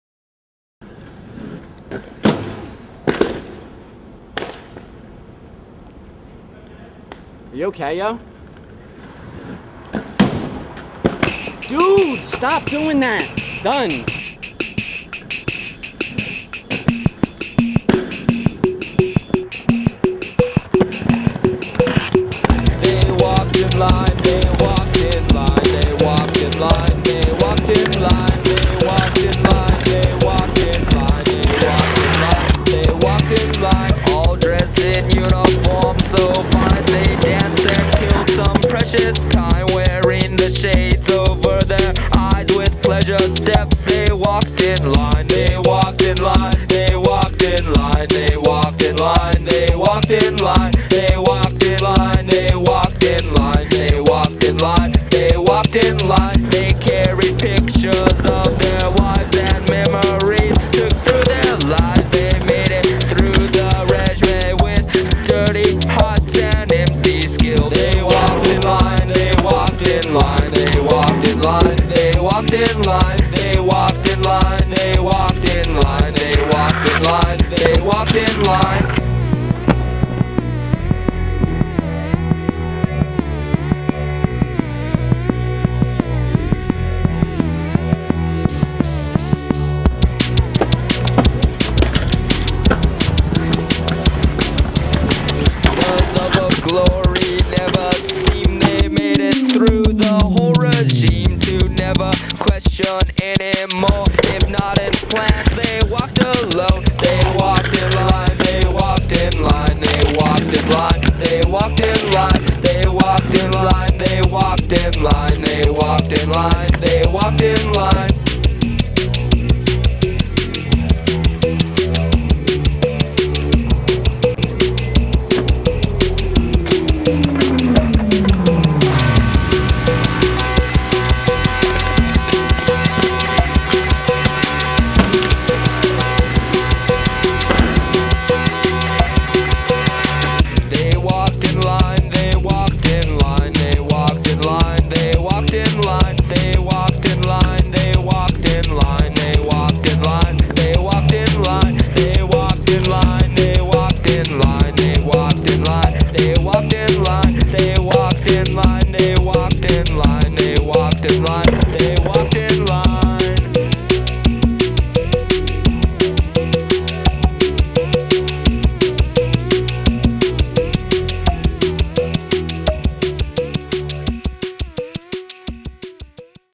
Colin Kelso - Truth II Leftovers
After filming for the Truth II for over a year and some now, everyone who is a part of the project has been stacking clips like madmen. This is brother Colin's give aways that just aren't up to par. Enjoy.